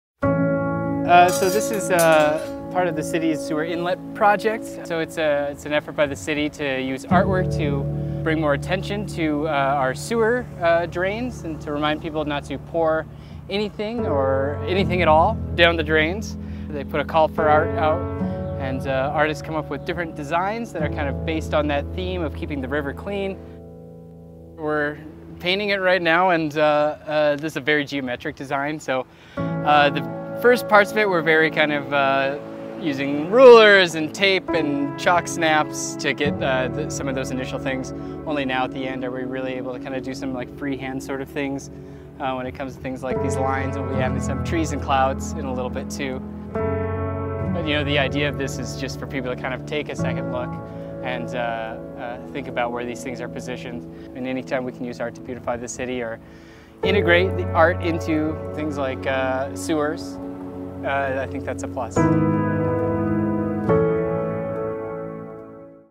City News - Storm Drain Art
The City of Sioux Falls is calling on local artists to help beautify downtown Sioux Falls. The Downtown Storm Inlet Art Project provides a unique canvas for public art, as well as an opportunity to highlight the need to protect our water systems.